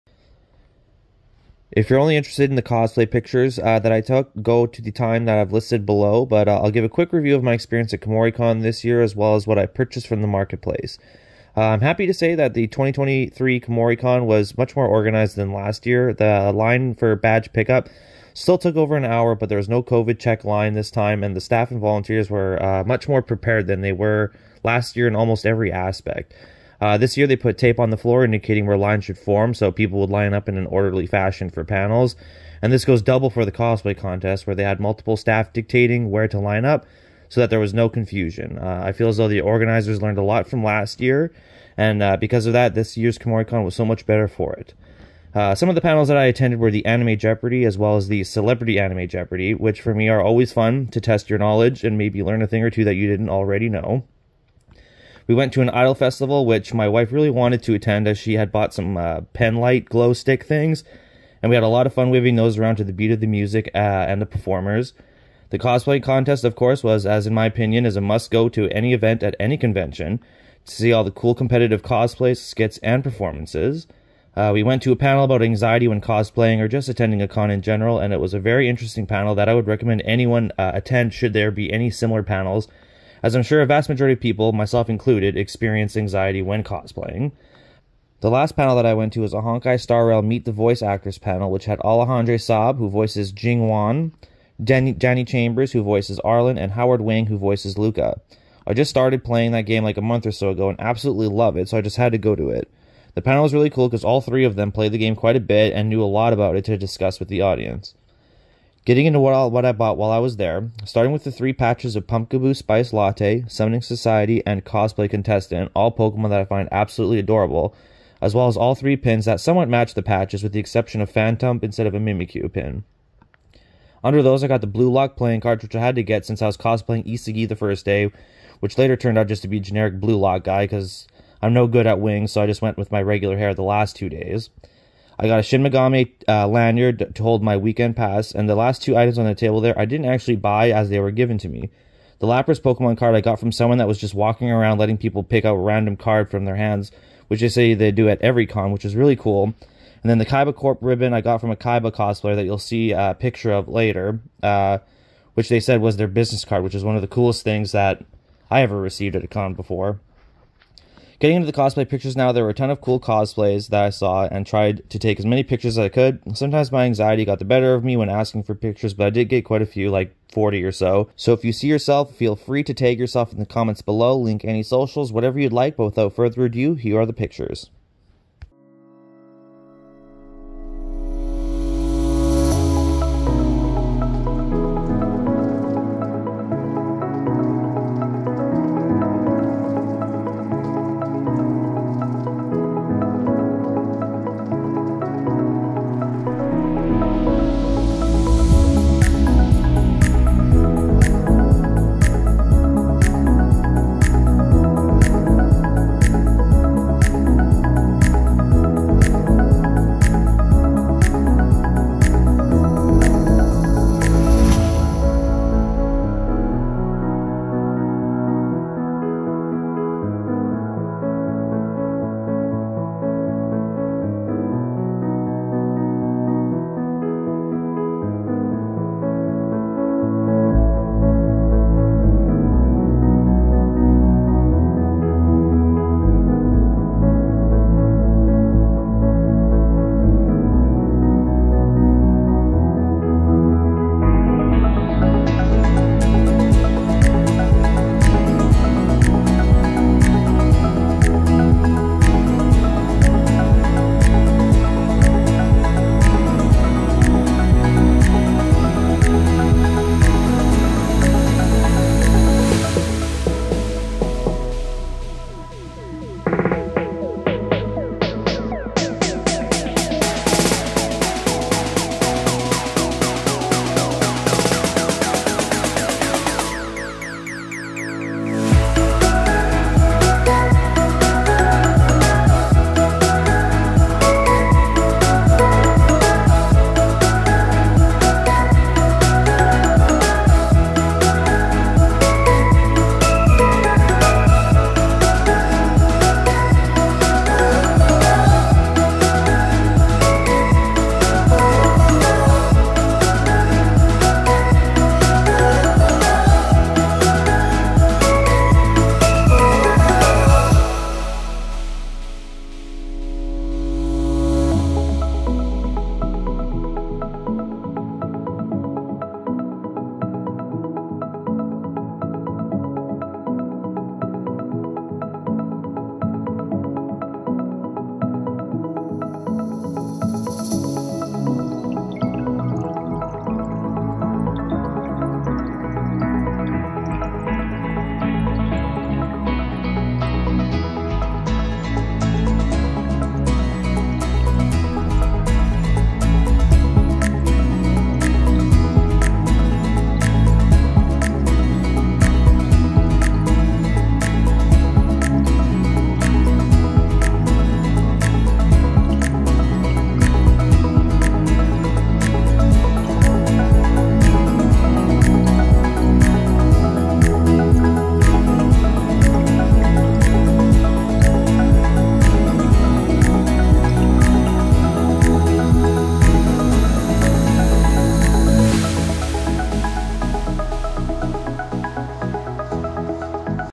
Kumoricon 2023 Anime Convention - Cosplay And My Experience
Kumoricon 2023 Anime Convention in Portland Oregon! I detail my experience at Kumoricon this year as well as show off the goodies I bought and the cosplay pictures I took! Feel free to tag yourself and your socials if you're in the video!